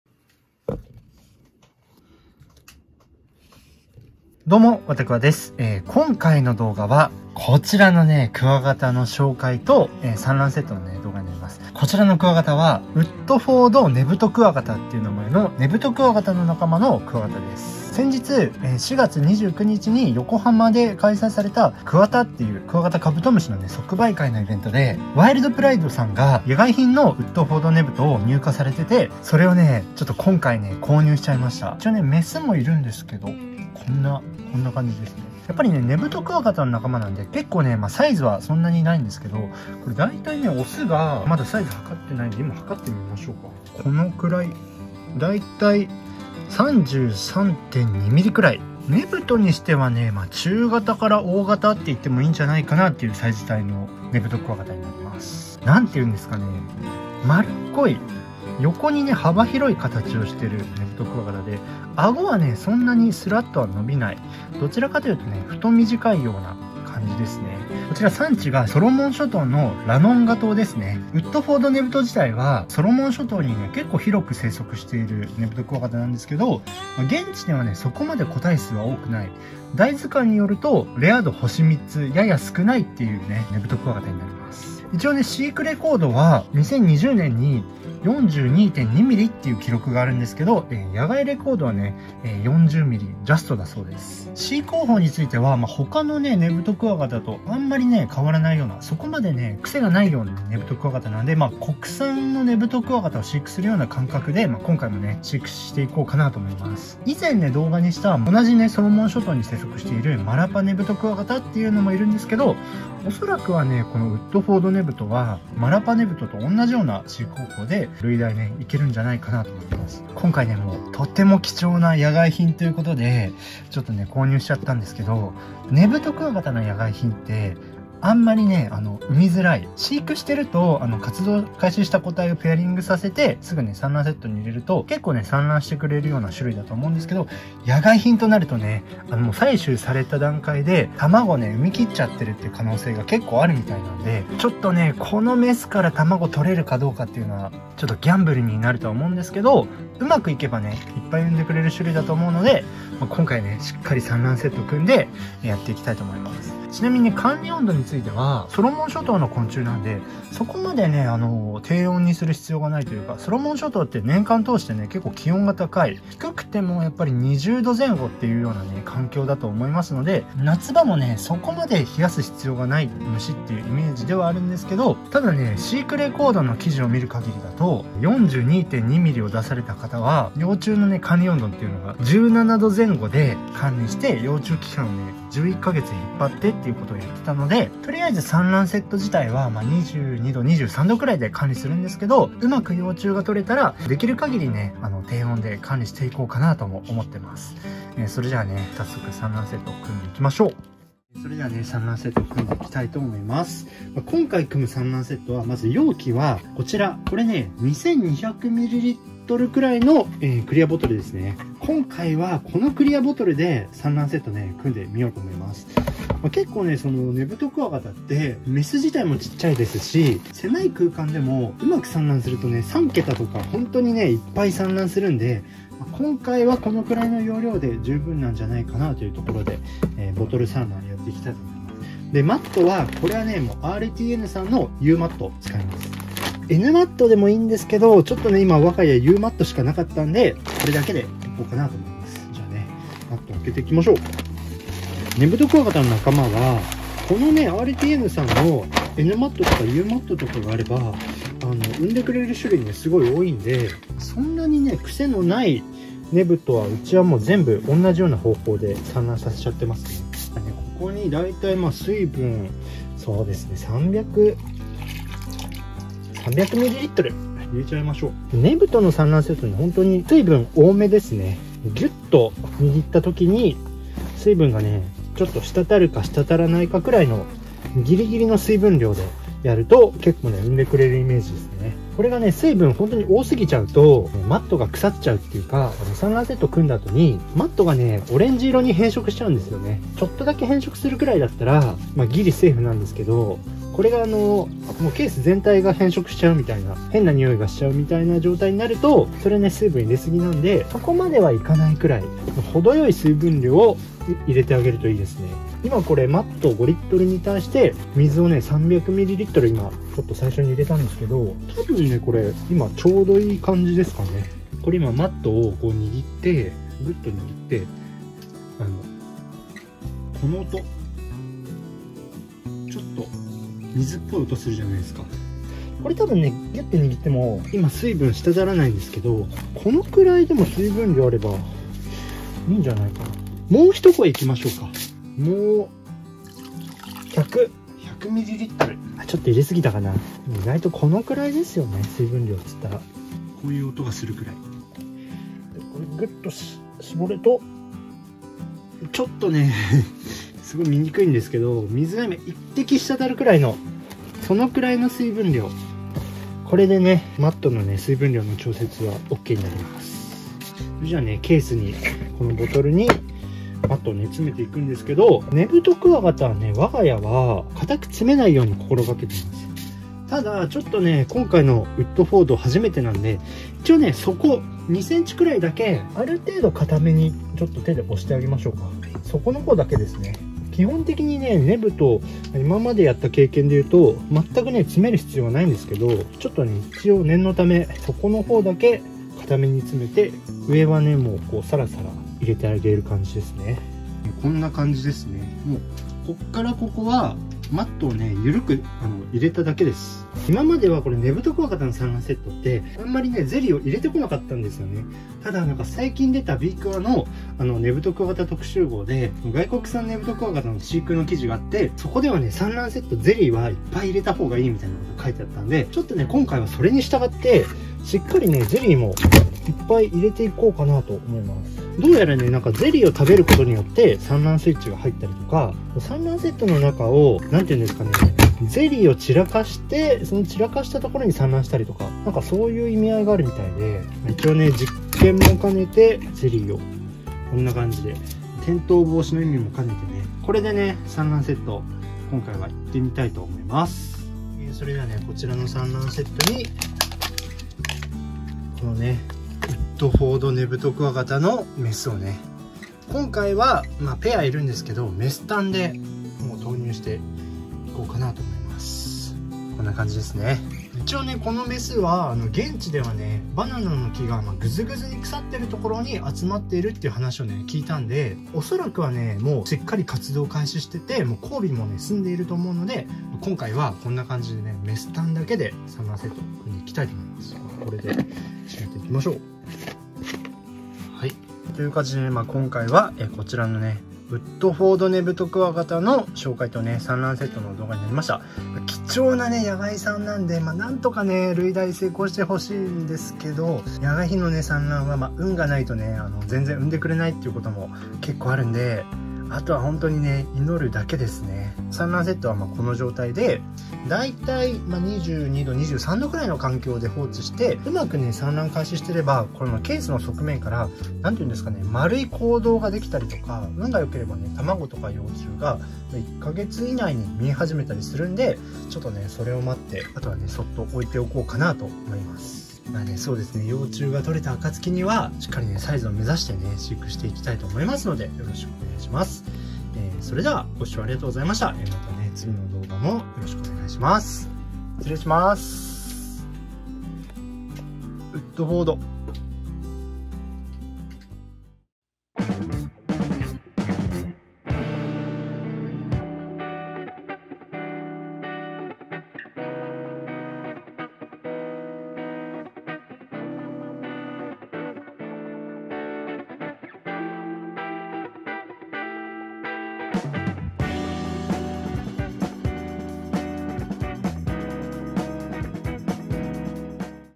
ウッドフォードネブトクワガタの産卵セット【クワガタ飼育】
今回の動画は、ウッドフォードネブトクワガタの紹介と産卵セットの動画です。

※この動画での内容は必ずしも正しいとは限りませんので参考にされる場合は自己責任でお願いします。